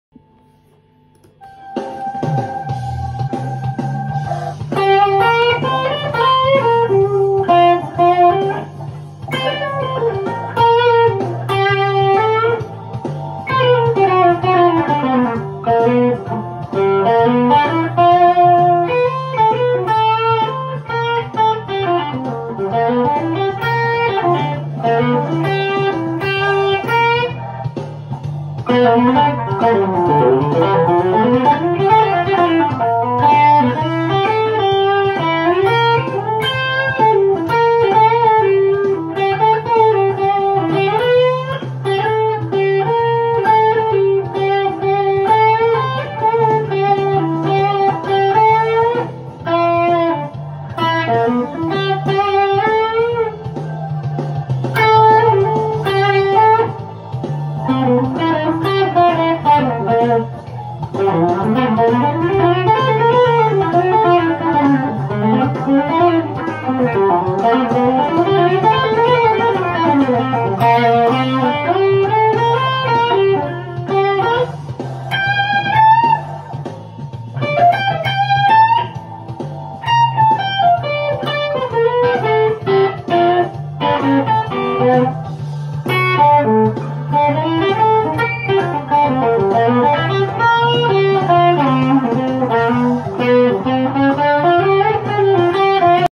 Gibson 339 2019 faded lightburst
Demo of the 2019 Gibson 339, Matchless Avalon and Suhr shiba drive. Killer tone man